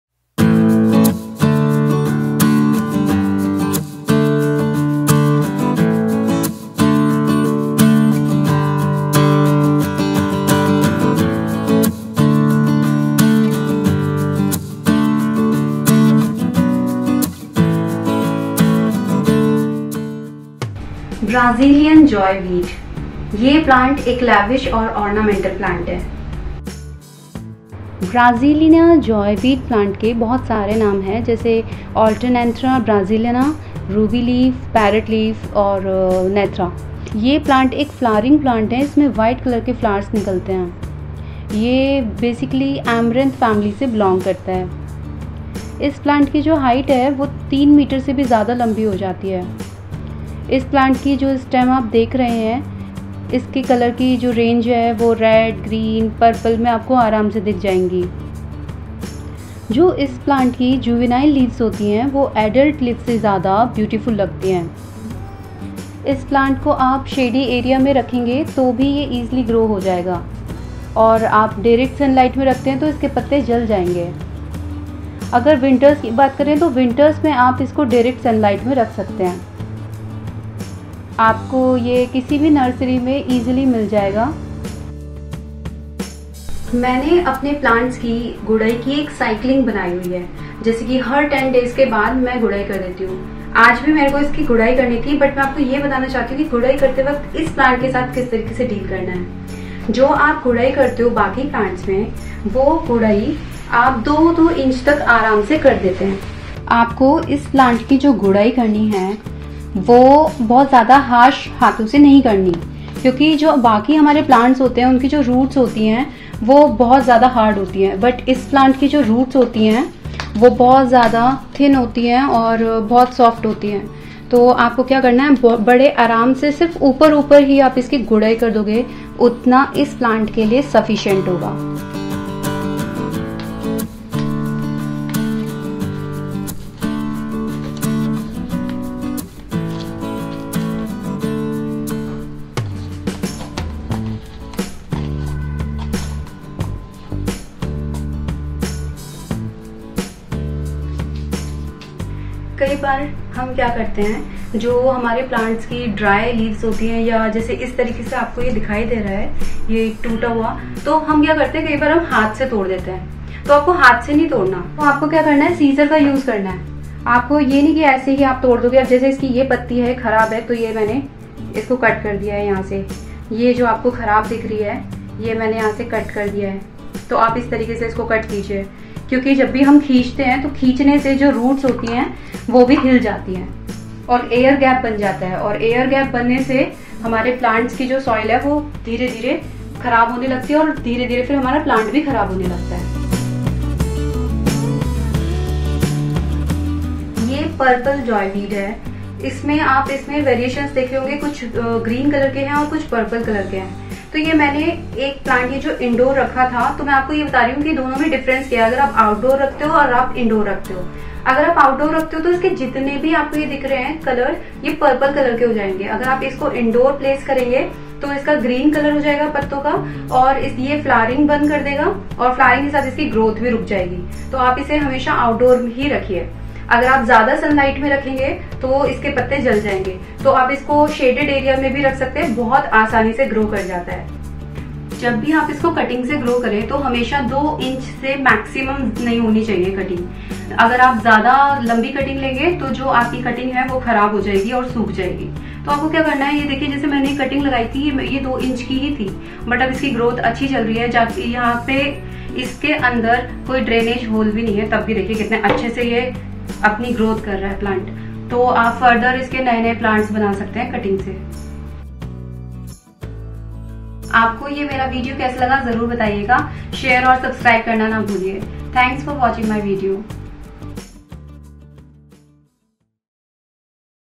Facts about Brazilian Joyweed
Also known as:  Joy Weed, Purple Alternanthera, Brazil Chaff-Flower, Purple-Leafed Chaff-Flower, Parrot Leaf, Purple Joyweed, Ruby Leaf, Copperleaf, Alligator Weed etc...
The Brazilian Joyweed is best known for its dark purple foliage. Its narrow leaves are rather elliptic to oval and a deep red-purple or, sometimes, when receiving less than full sun, purple-green in color.  The more sun it gets, the darker purple it the leaves become.